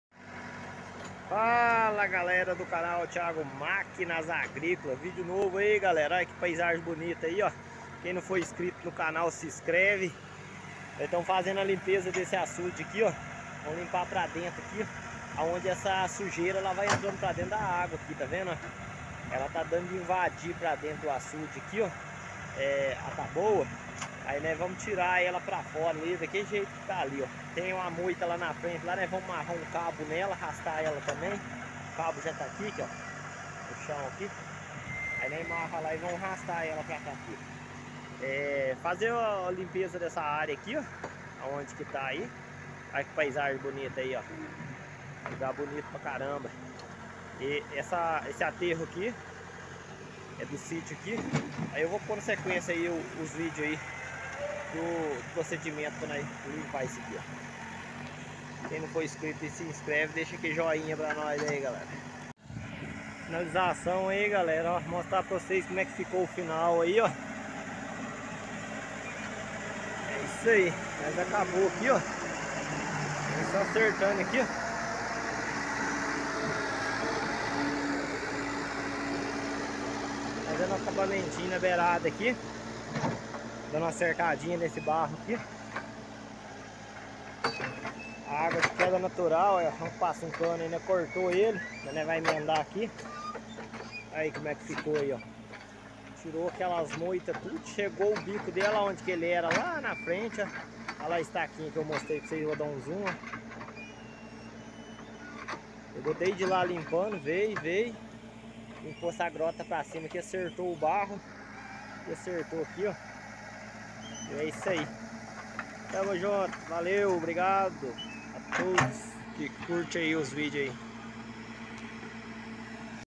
fazendo limpeza de açude com a retroescavadeira Case 580l
arrancando moita do açude com a retroescavadeira Case 580l fazendo limpeza de açude